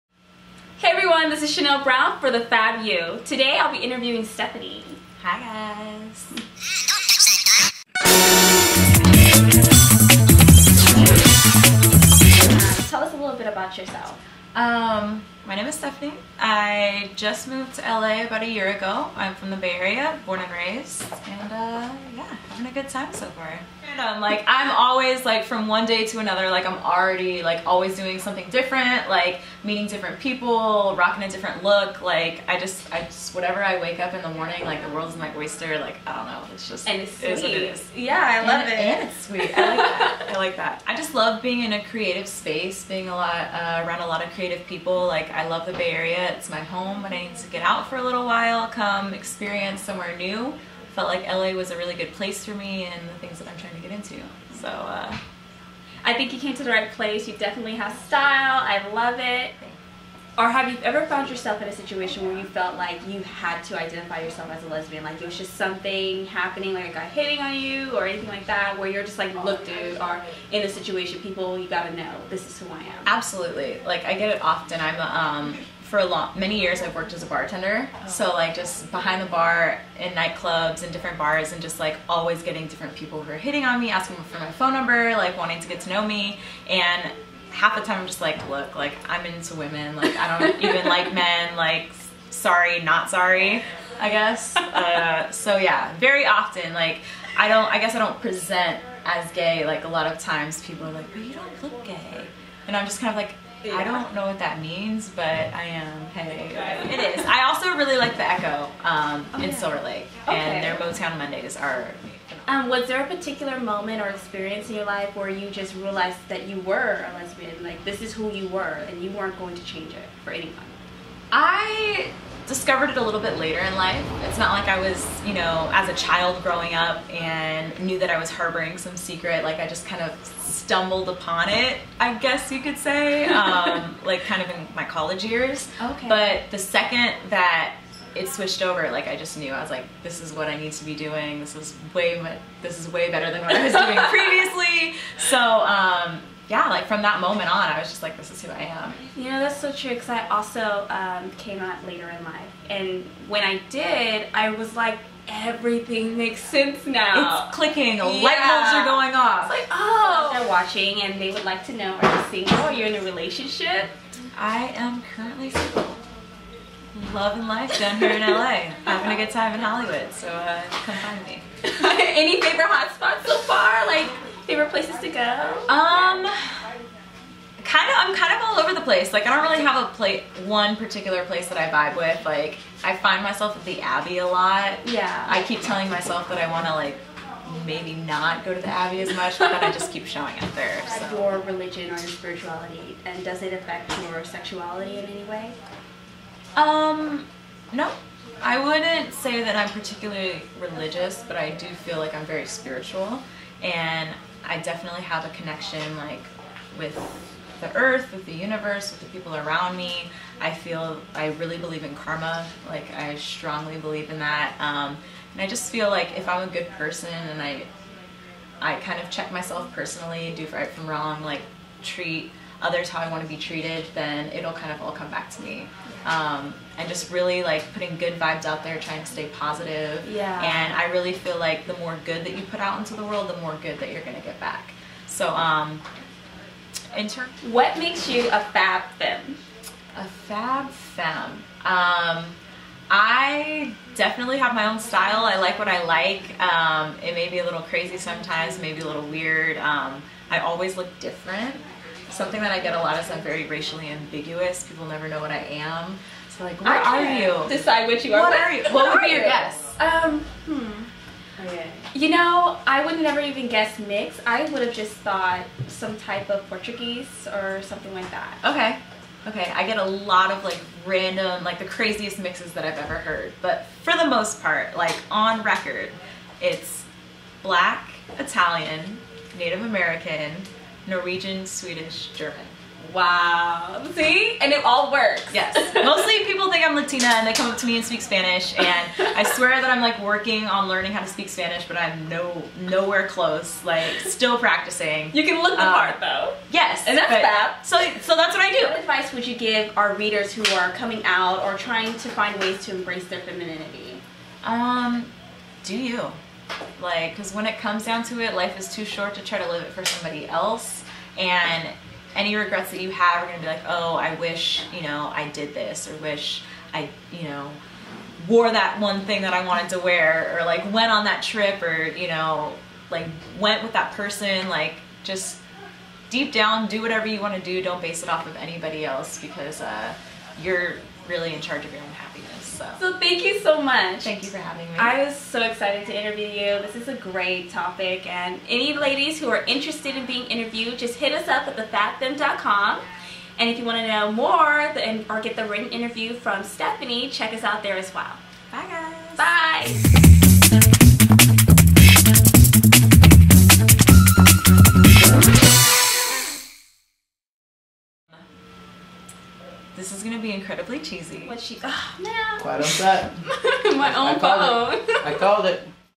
The Fab You: Stephanie
Chanel Brown interviews amazing women just like you in this video blog series of The Fab You. Join us in embracing your fab inner being!